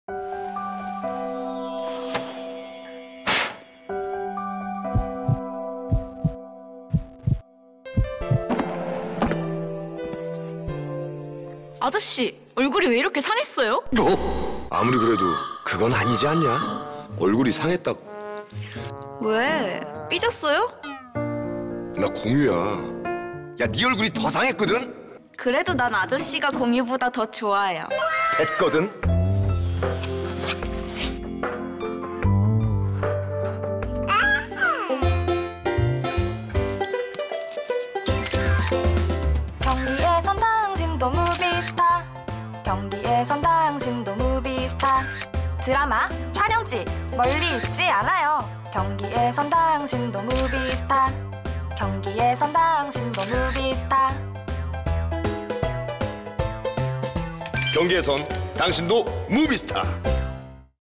[경기에선 당신도 무비스타 4편] 드라마 도깨비_안성 미리내 성당
경기에선 당신도 무비스타 4편

다양한 영화 촬영지 및 관광지 배경으로 사진 촬영,
경기도에서는 당신도 무비스타가 될 수 있어요!!